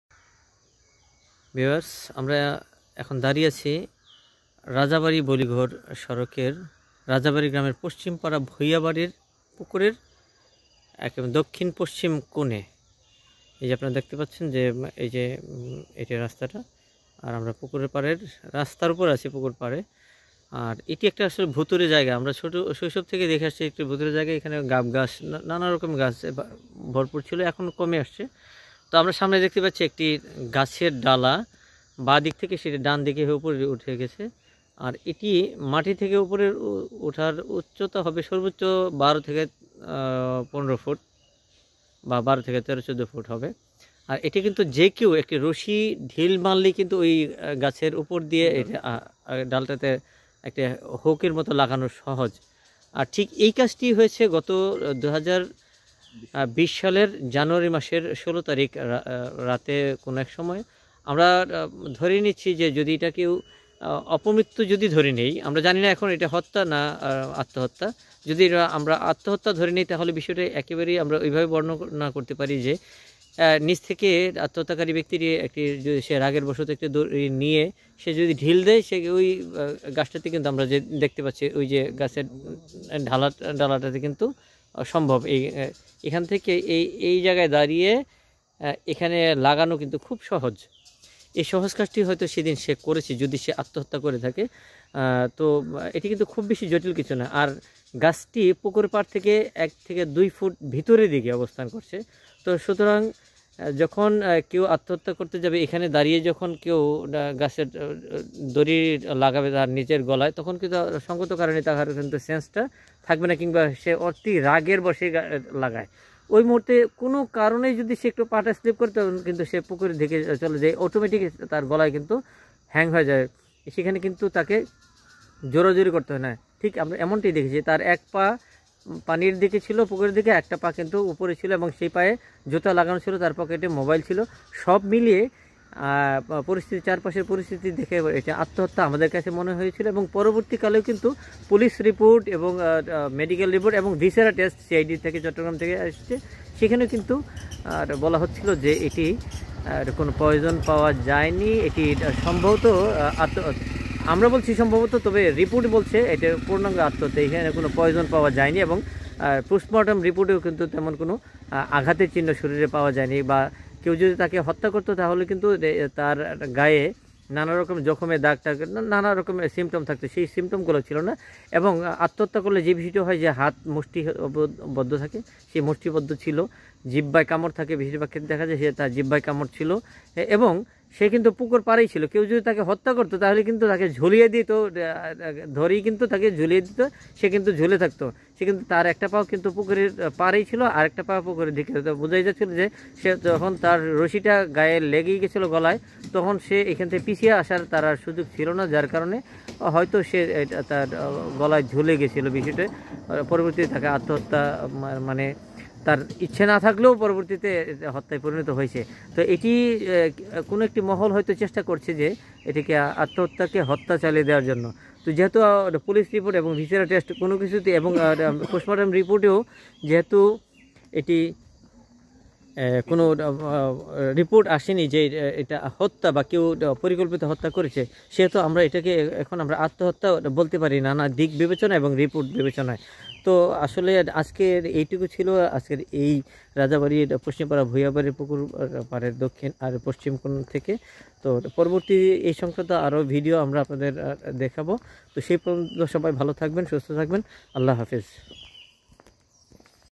রহস্যের খোঁজে রাজাবাড়ির সেই ভুতুড়ে পুকুরপাড়ে ।n search of the mystery in that haunted pond Rajabari
রহস্যের খোঁজে রাজাবাড়ি গ্রামের সেই ভুতুড়ে পুকুরপাড়ে।

In search of the mystery in that haunted pond of Rajabari village.